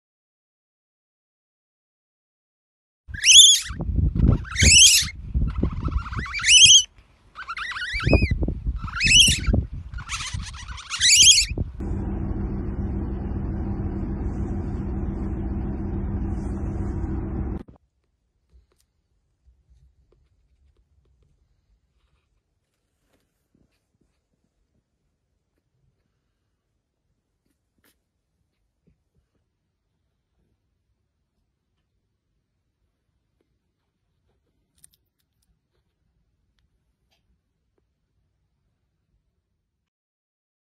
Otter cub loses mum and travels by ferry to an animal sanctuary
A little otter cub has been rescued from the Isle of Yell after losing its mother.

A spokesperson from the Hillswick Wildlife Sanctuary said: “Keith Nisbet was slowly turning a corner at Ulsta when an otter darted in front of his minibus.

“He slammed on the brakes sure that he had hit the creature, but when he climbed out to look he found the otter had disappeared leaving behind her tiny cub.

The minibus driver moved the otter cub to the side of the road hoping the mother would return but unfortunately the mum didn’t come back.

The little cub is now in the care of experts at Hillswick Wildlife Sanctuary.